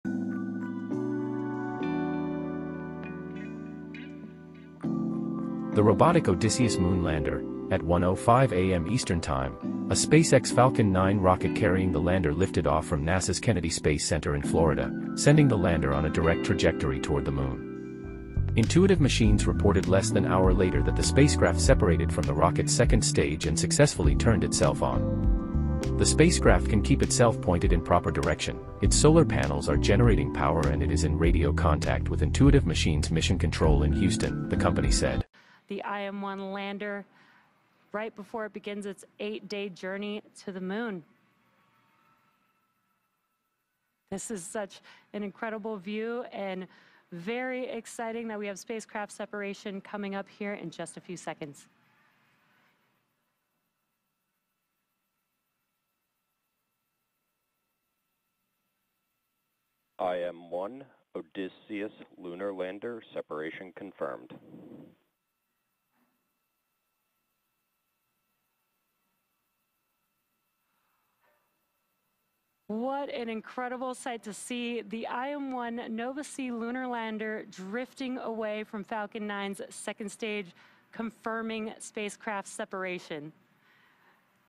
IM-1 lunar lander in amazing view from space || IM-1 Lunar Landing awaiting
Intuitive Machines-1 Odysseus moon lander.
IM-1 Launch to the Moon!
The robotic Odysseus moon lander, a SpaceX Falcon 9 rocket carrying the lander lifted off from NASA’s Kennedy Space Center in Florida, sending the lander on a direct trajectory toward the moon.
If all goes well, in nine days, it will become the first American spacecraft to gently set down on the moon’s surface since the Apollo 17 moon landing in 1972.